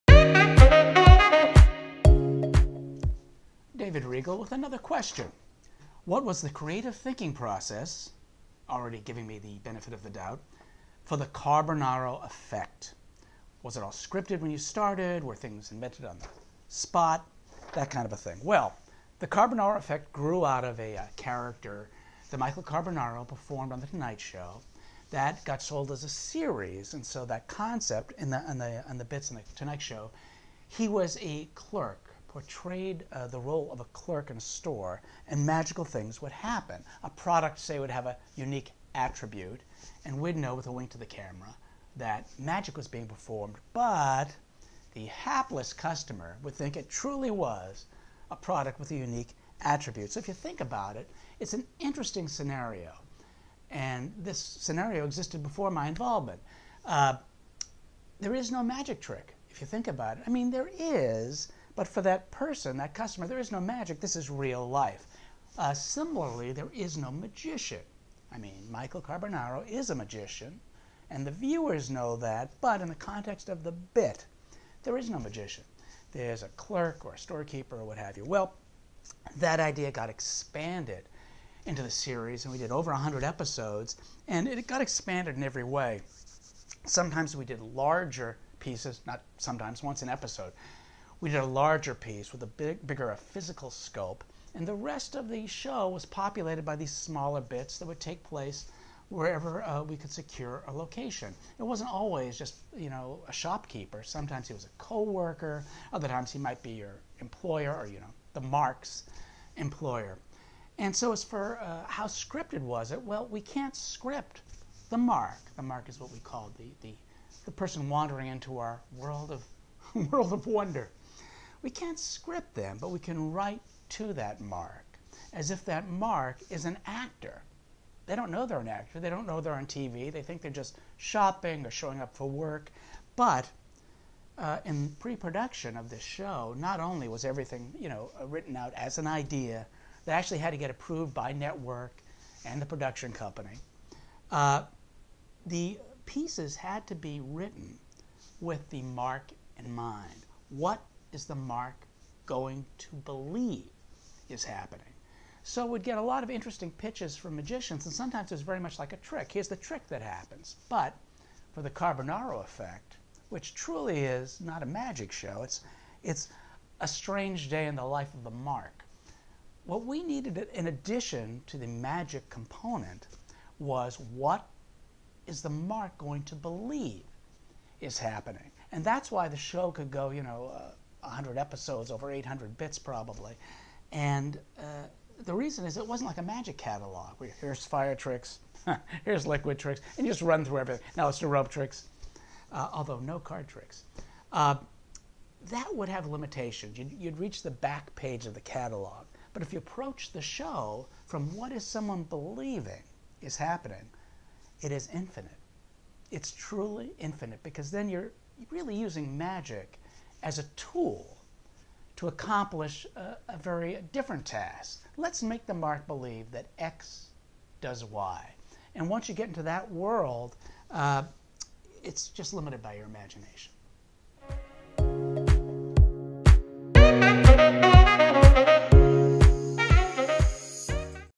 TrickTribe TV - AQAAM #1 - David Regal on the Carbonaro effect
TrickTribe invite its followers to submit us a question they wish to ask to any magician. Hence AQAAM = Any Question At Any Magician.

Here David Regal gets asked about the process behind the Carbonaro Effect!